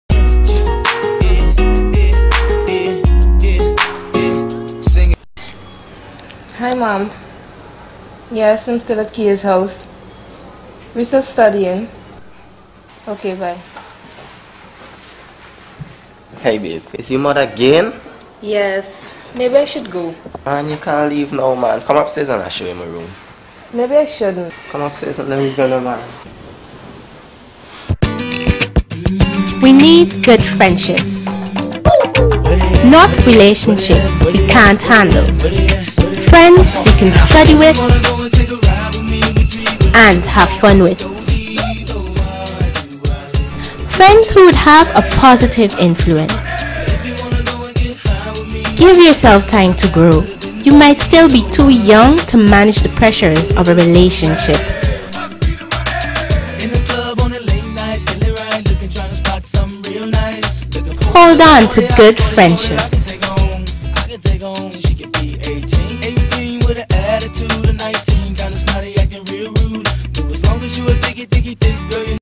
Child Rights.mp4
A Public Service Announcement done by Rights of Children ( ROC) on Good Friendships.